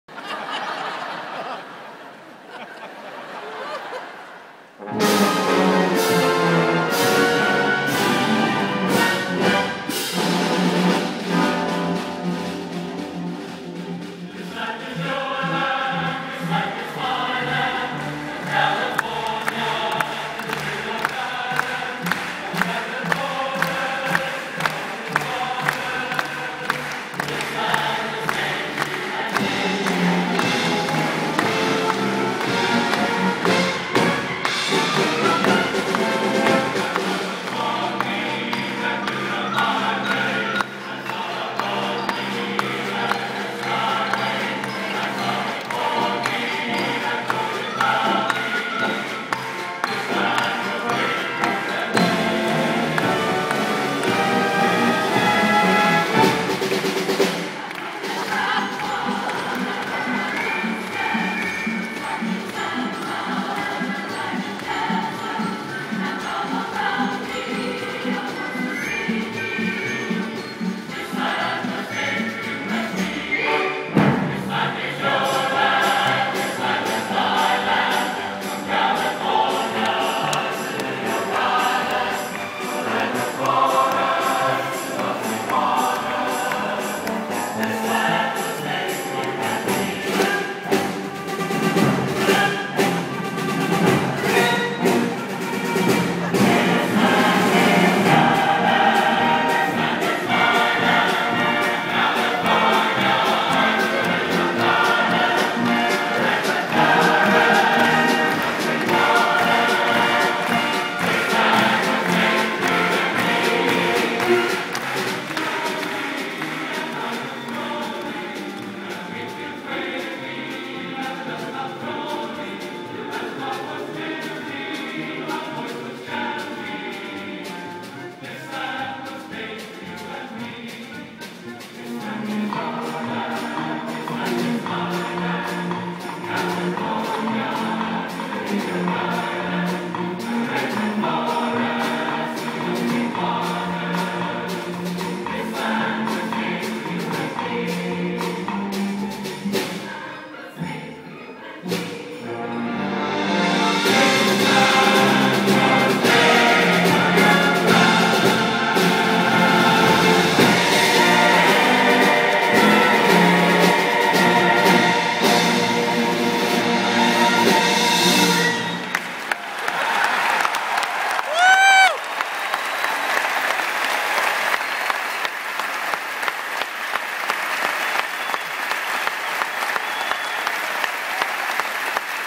Senator Mark Warner conducts the Mormon Tabernacle Choir
Concert this evening at Wolf Trap National Park for the Performing Arts in Vienna, Virginia.